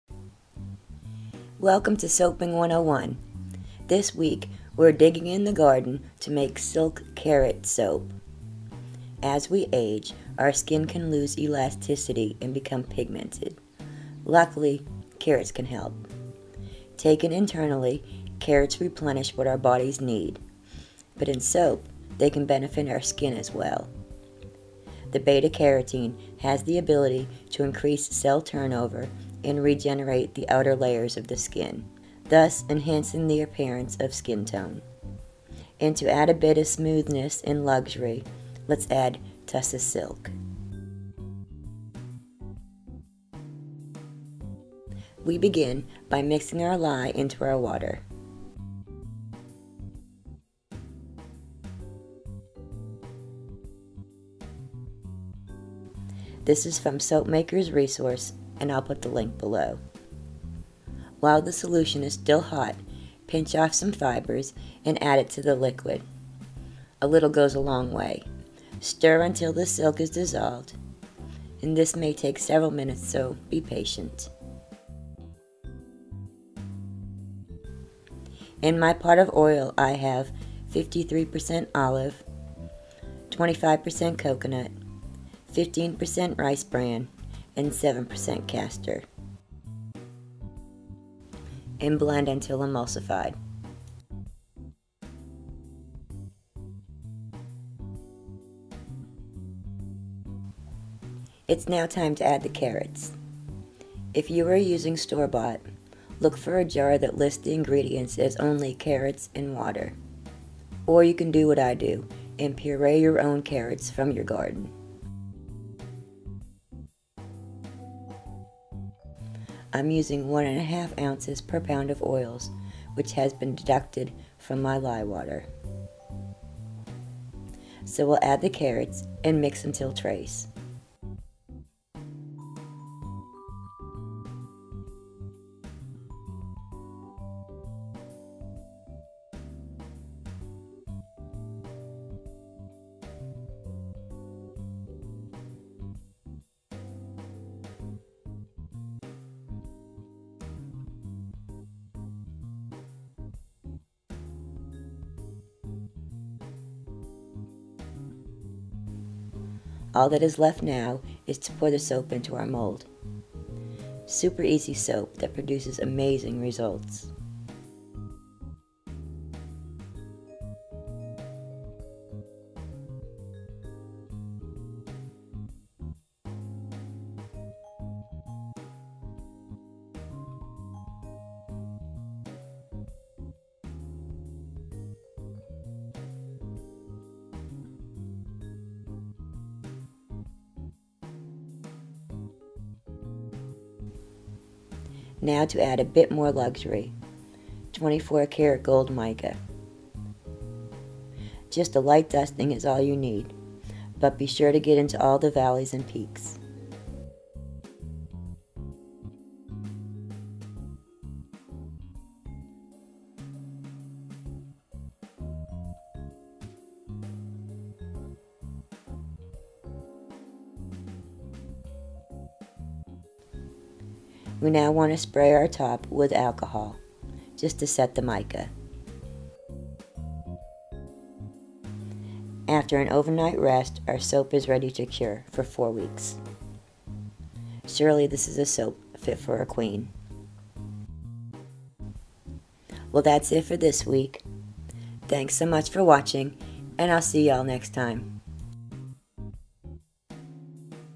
How to Make Carrot Soap {with silk} S2W10
Season 2 Week 10 of Soaping101 and we are creating a luxury soap with fresh carrots and tussah silk.